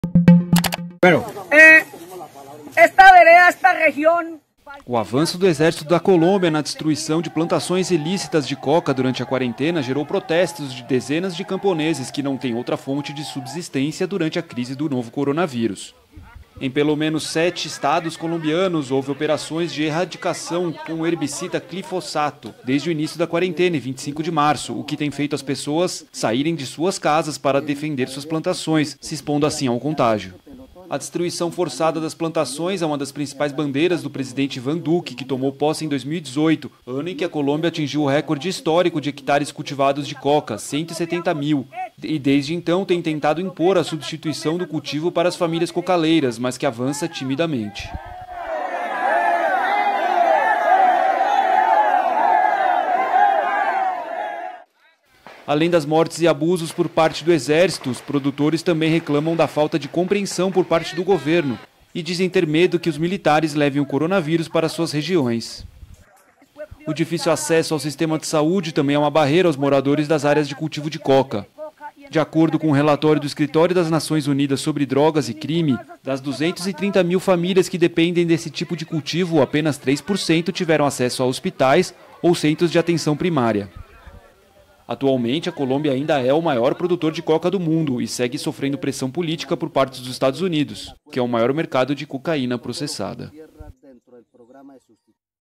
Destruição de campos de coca na Colômbia eleva tensão entre plantadores e o exército
Bogotá (Colômbia), 14 de maio (EFE) (Imagens cedidas: COCCAM e Ascamca) - O avanço do exército da Colômbia na distruição de plantações ilícitas de coca durante a quarentena gerou protestos de dezenas de camponeses que não têm outra fonte de subsistência durante a crise do novo coronavíris.

Em pelo menos sete estados colombianos houve operações de erradicação com o herbicída glifosato desde o início da quarentena, em 25 de março, o que tem feito as pessoas a saírem de suas casas para defender suas plantações, se expondo ao contágio.

A erradicação forçada das plantações é uma das principais bandeiras do presidente Iván Duque, que tomou posse em 2018, ano em que a Colômbia atingiu o recorde histórico de hectares cultivados de coca - 170 mil - e desde então tem tendo impor a substituição de cultivo para as famílias cocaleiras, mas que avança timidamente.